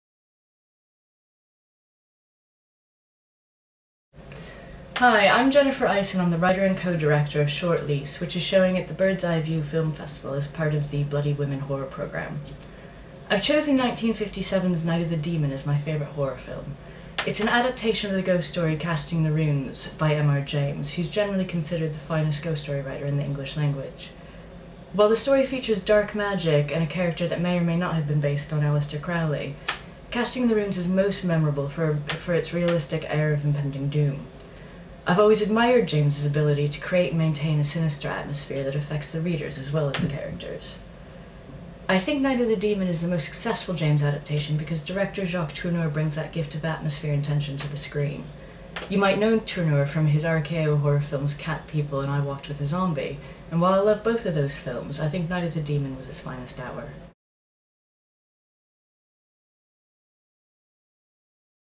Horror Epiphany with Jennifer Eiss
Writer and director Jennifer Eiss introduces her favourite horror film: Jacques Tourneur's 1957 classic Night of the Demon. Click on the link in the video to watch the film on MUBI.

Birds Eye View and MUBI have teamed up to present Horror Epiphanies - interviews with the writers and directors who helped put together Birds Eye View's Bloody Women programming exploring women in horror.

For full details of the Birds Eye View Film Festival 2011, from 8 - 17 March,